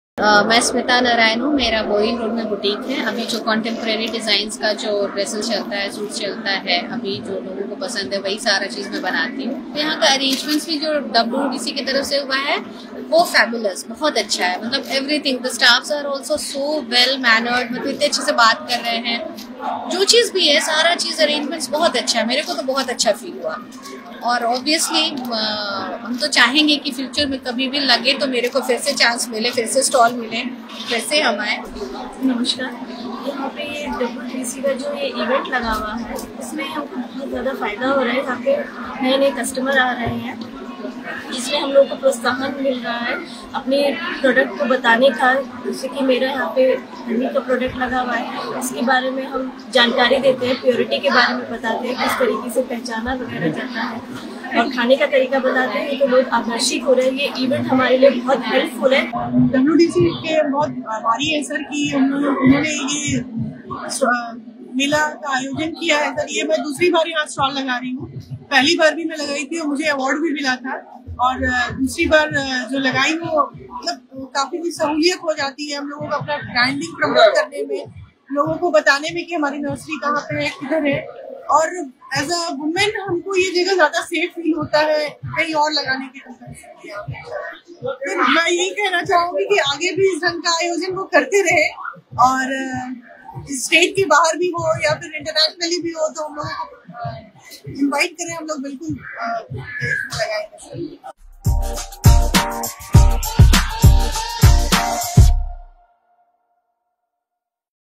स्मिता नारायण - महिला उद्यमी मेला 2021
महिला उद्यमी मेला 2021 में महिला उद्यमियों को मिला एक बेहतरीन मंच
🗓️ 14-18 जनवरी 2021
⏰ 10:00 AM - 7:00 PM
📍ज्ञान भवन, गांधी मैदान, पटना